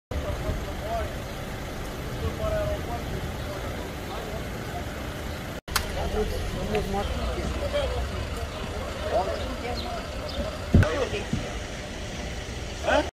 Constanța. Accident rutier cu o victimă la intersecția străzilor Atelierelor și Constantin Brătescu
O persoană a fost rănită în urma evenimentului rutier. Cadrele medicale au fost solicitate să intervină în această după-amiază, în municipiul Constanța.

Conform primelor informații oferite de ISU „Dobrogea”, s-a produs un accident rutier între două autoturisme la intersecția străzilor Atelierelor și Constantin Brătescu